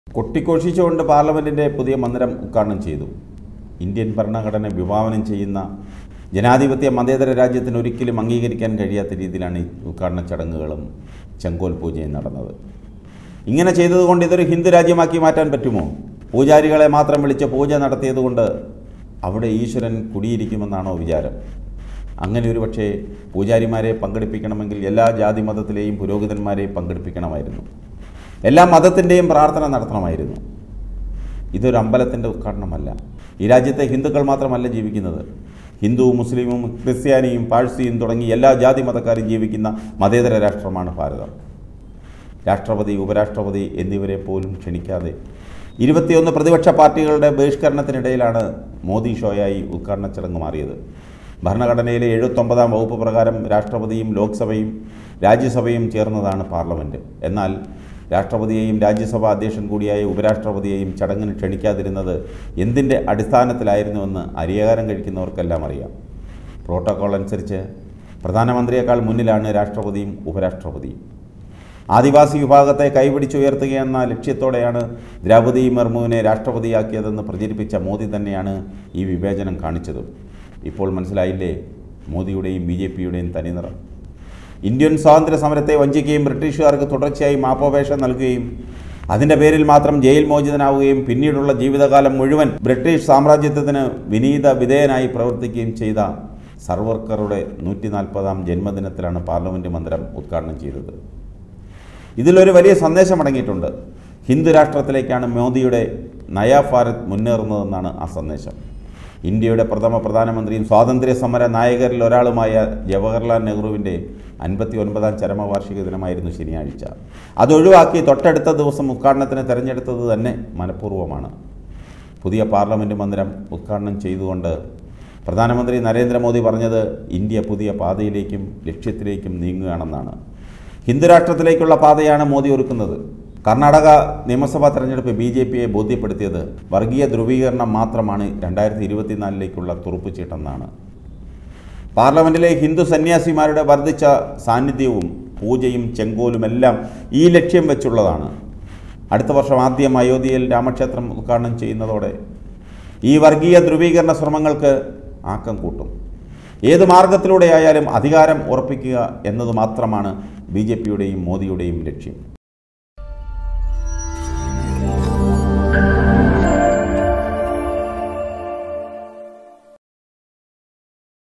ലോകത്തിലെ മുഴുവൻ പൂജാരിമാരെ കൊണ്ടുവന്ന് പൂജിച്ചാലും ഇതൊരു ഹിന്ദു രാഷ്ട്രമാവില്ല
Tag-Generator-Youtube-malayalam news-global